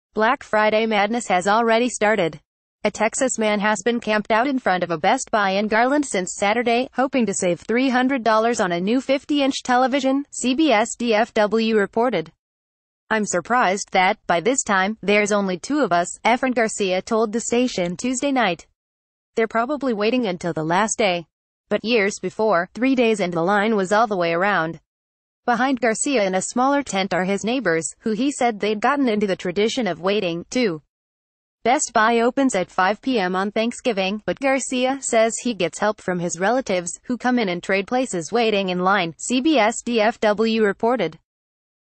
The Fox News - Black Friday sales 2017 prompted texas campers since Saturday
The Fox News - The Fox News - Black Friday sales 2017 prompted texas campers since Saturday

Black Friday madness has already started. A Texas man has been camped out in front of a Best Buy in Garland since Saturday, hoping to save $300 on a new 50inch television, CBS DFW reported. “I’m surprised that, by this time, there’s only two of us,” Efren Garcia told the station Tuesday night. “They’re probably waiting until the last day. But years before, three days and the line was all the way around.” Behind Garcia in a smaller tent are his neighbors, who he said theyd gotten into the traditi...